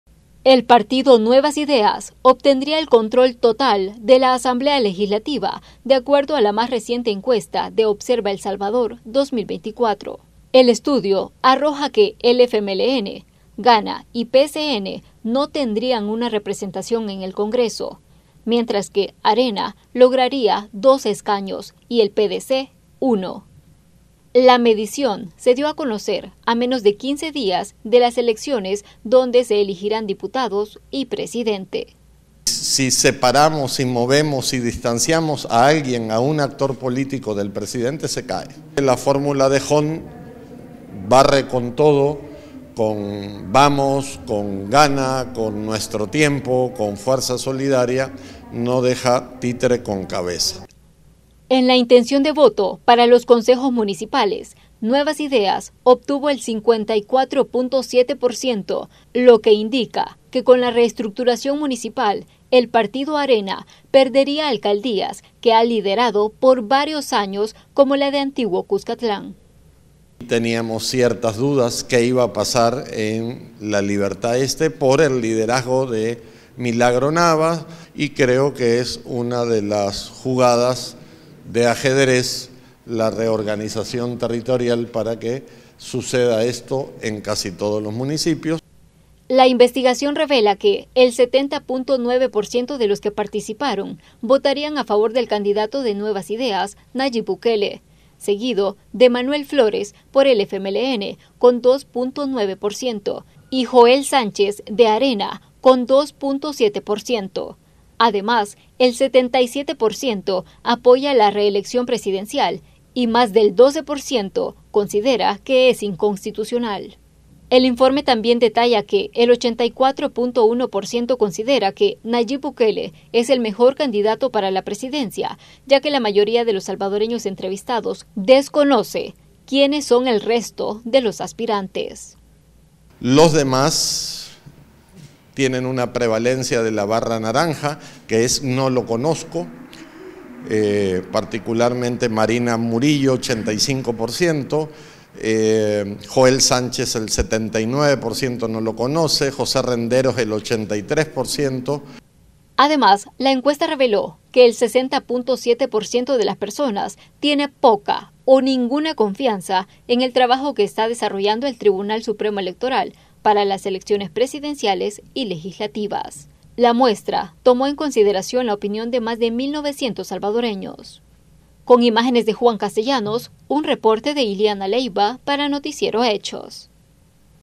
Observa El Salvador presento los resultados de su encuentra de perspectivas electorales
Conozca como quedaría conformada la Asamblea Legislativa luego de los comicios, según una reciente encuesta.